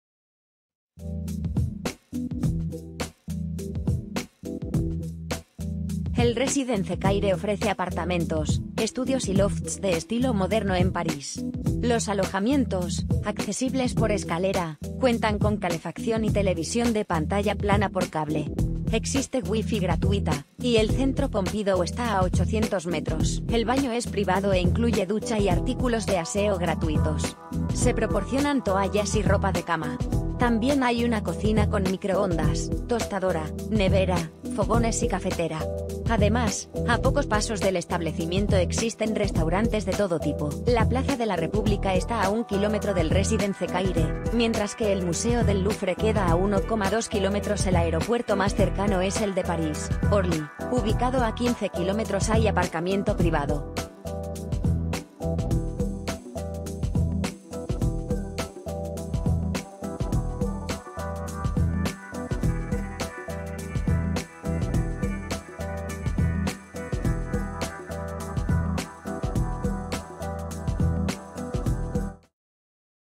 Résidence Caire / Montorgueil |Comentarios de clientes. Opiniones reales. París, Francia
El Résidence Caire ofrece apartamentos, estudios y lofts de estilo moderno en París. Los alojamientos, accesibles por escalera, cuentan con calefacción y TV de pantalla plana por cable. Existe WiFi gratuita, y el Centro Pompidou está a 800 metros. El baño es privado e incluye ducha y artículos de aseo gratuitos. Se proporcionan toallas y ropa de cama. También hay una cocina con microondas, tostadora, nevera, fogones y cafetera. Además, a pocos pasos del establecimiento existen restaurantes de todo tipo. La Plaza de la República está a 1 km del Résidence Caire, mientras que el Museo del Louvre queda a 1,2 km. El aeropuerto más cercano es el de París - Orly, ubicado a 15 km. Hay aparcamiento privado.